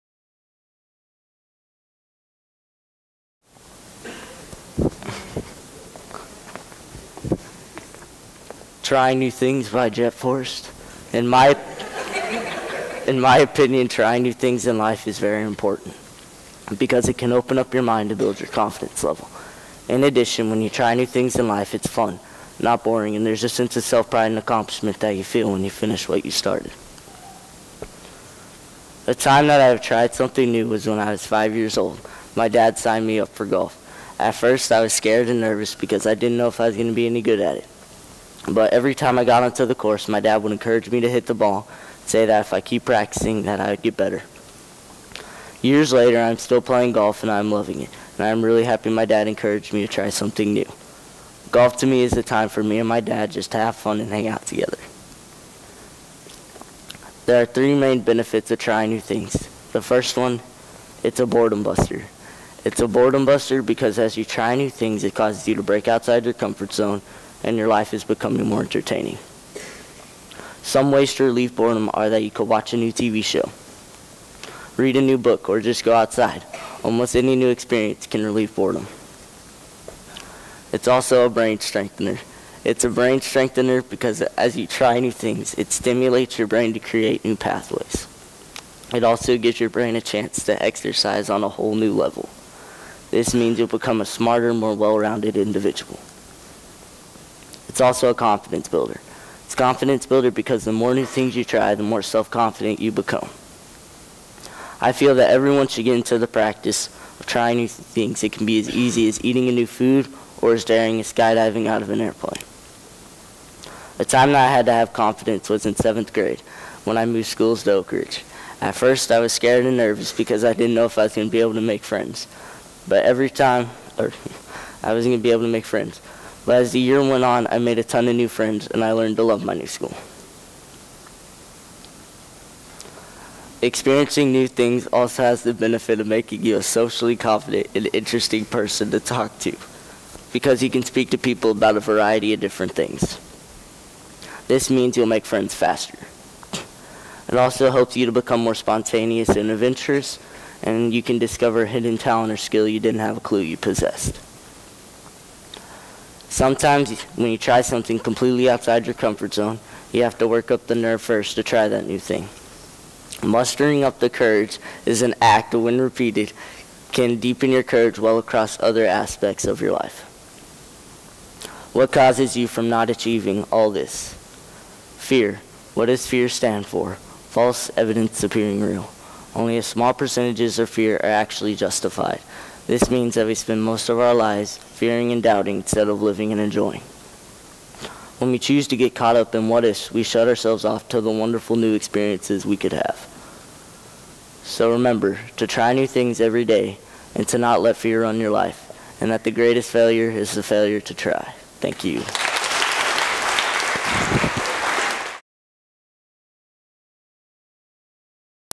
Jett Forrest - "Trying New Things"
Oakridge 8th grader Jett Forrest presents his Capstone Speech on May 16th.